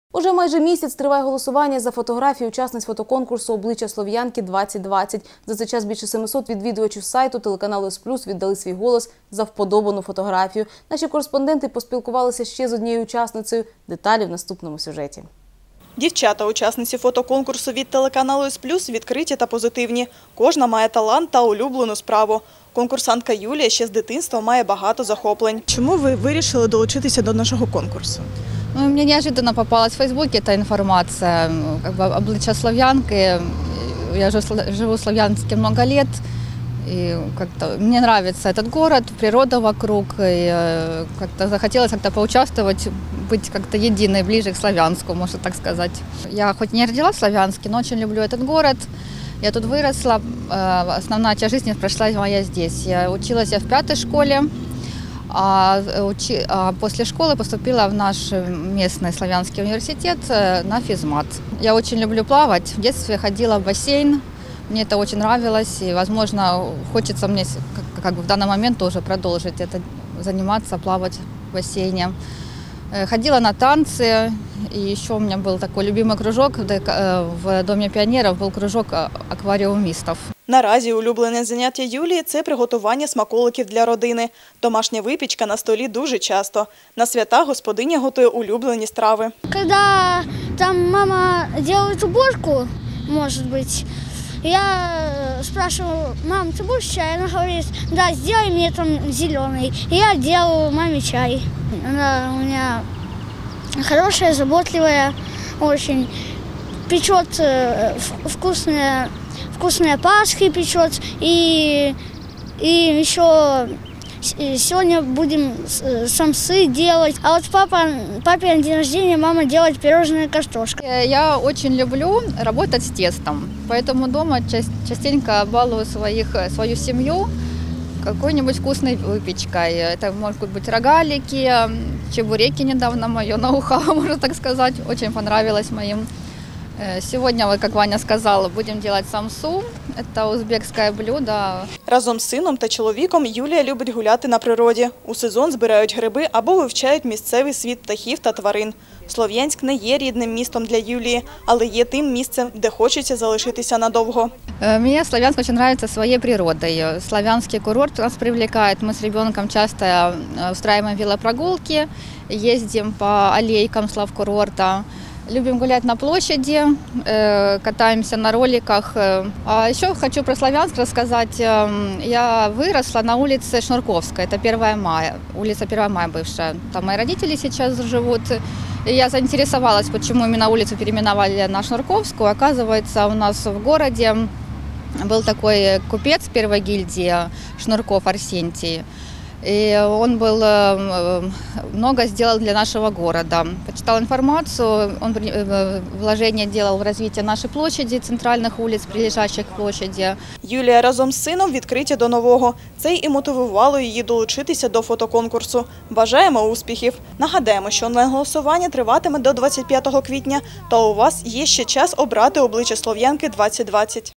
Уже майже місяць триває голосування за фотографії учасниць фотоконкурсу «Обличчя слов’янки - 2020».
Уже майже місяць триває голосування за фотографії учасниць фотоконкурсу «Обличчя слов’янки - 2020». За цей час більше 700 відвідувачів сайту телеканалу «С-плюс» віддали свій голос за вподобану фотографію. Наші кореспонденти поспілкувалися ще з однією учасницею